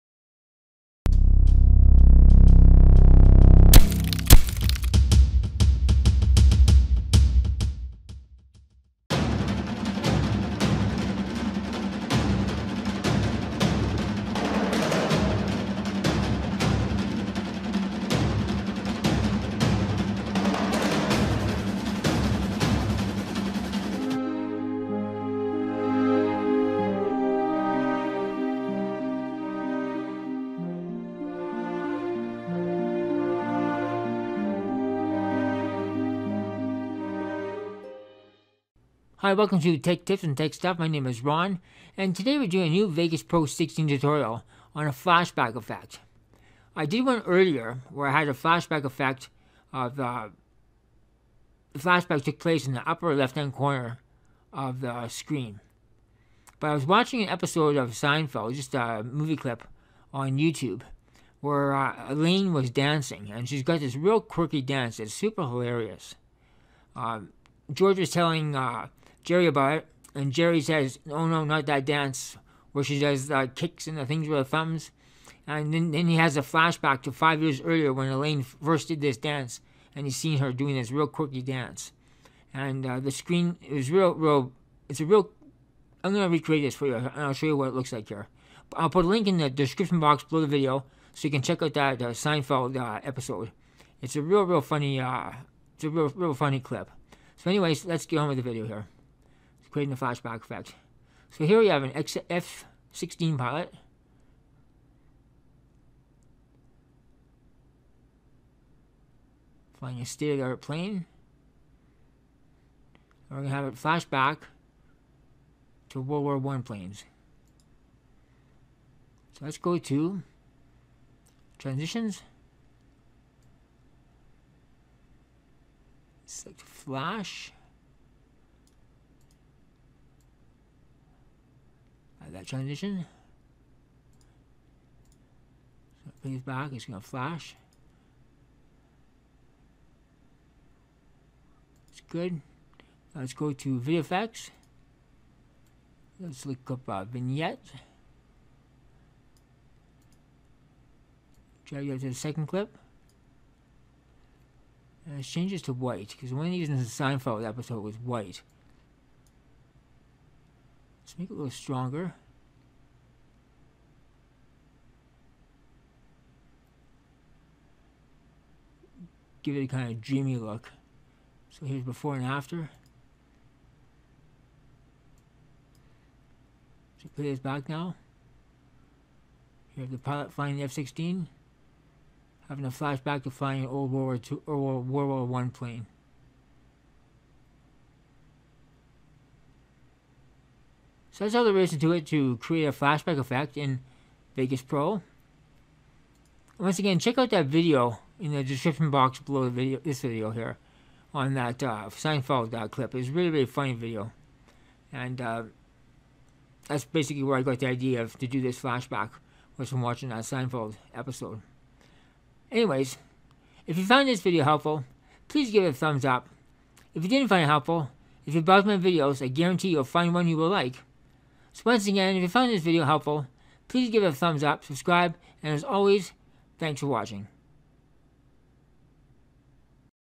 Seinfeld Inspired Flashback Effect Vegas Pro 16
In this tutorial I will show you how to create the Seinfeld inspired flashback effect in Vegas Pro.